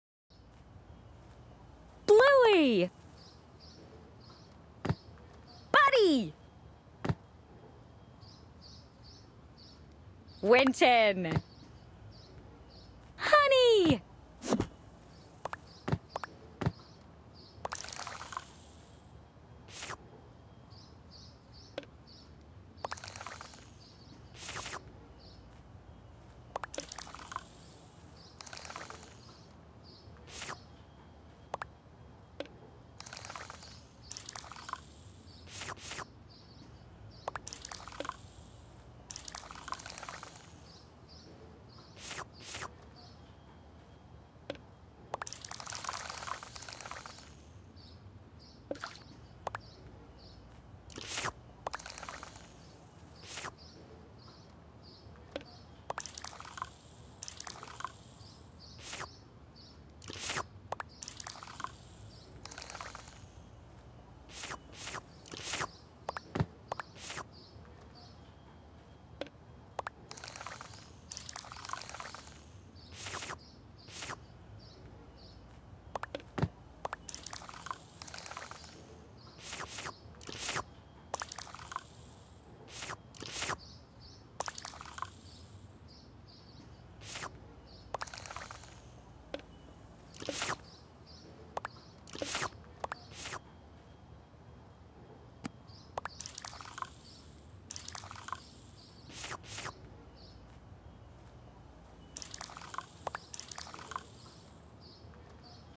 Bluey: Dark chocolate smoothie Party 🧋 02
Get ready to dive into a world of delicious fun at Bluey's Dark Chocolate Smoothie Party! Join Bluey and her friends for a scrumptious and exciting adventure filled with creamy, rich dark chocolate smoothies that will tantalize your taste buds. Perfect for kids and families, this event is designed to blend creativity, flavor, and laughter in one unforgettable experience.